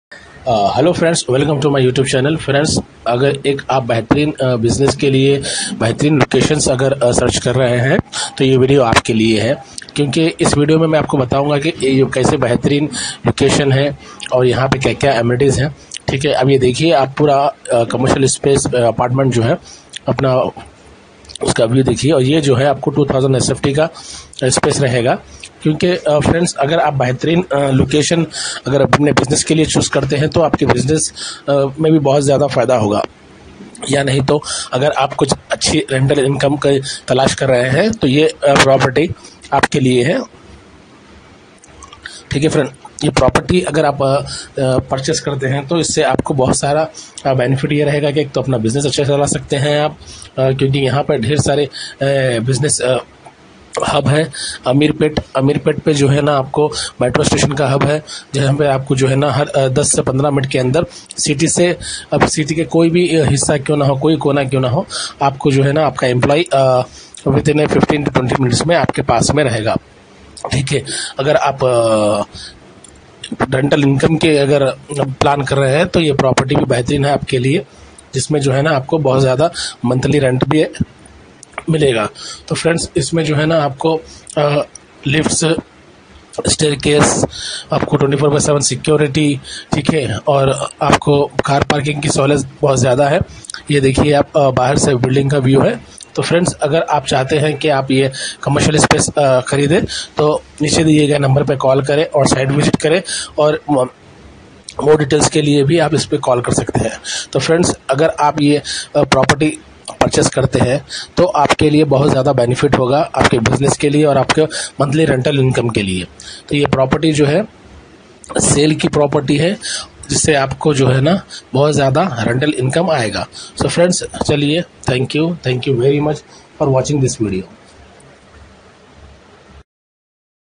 2000 Sft Commercial Space for Sale in Ameerpet, Hyderabad Prime Location Contact
Get affordable main road-connected commercial office space for sale in Ameerpet, Hyderabad at prime location connected to Nh-9 . Area of this space (2000 Sq.Ft) of Price 10500 per sft. with negotiable.